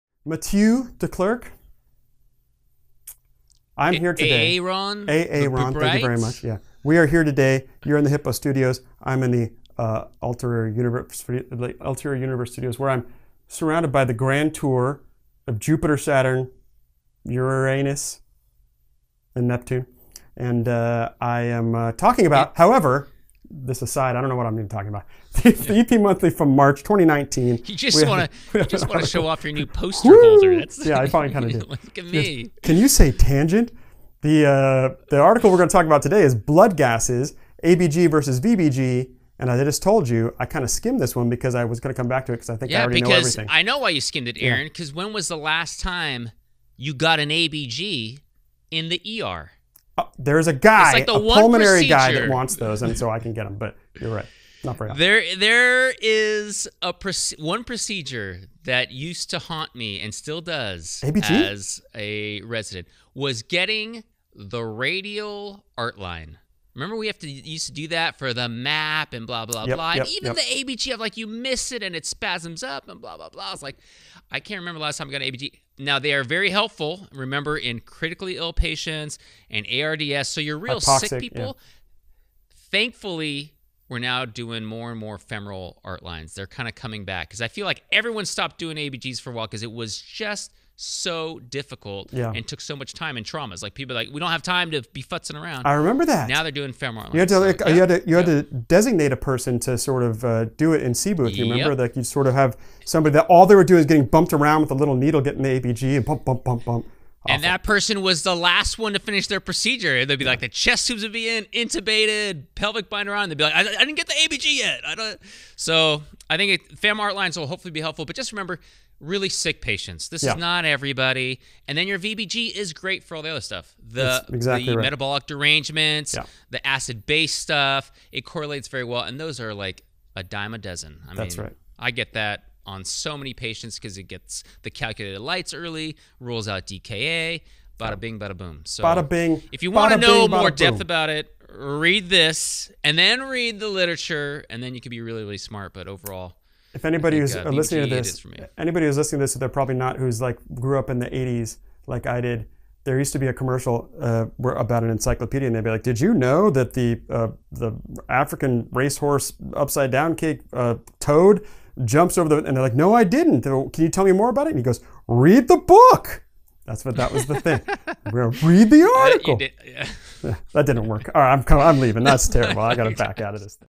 EPM March 2019 - Blood Gases
Aaron and Matt discuss the latest March 2019 EPM article exploring arterial and venous blood gases that provide crucial information about the acid-base status of critically ill ED patients.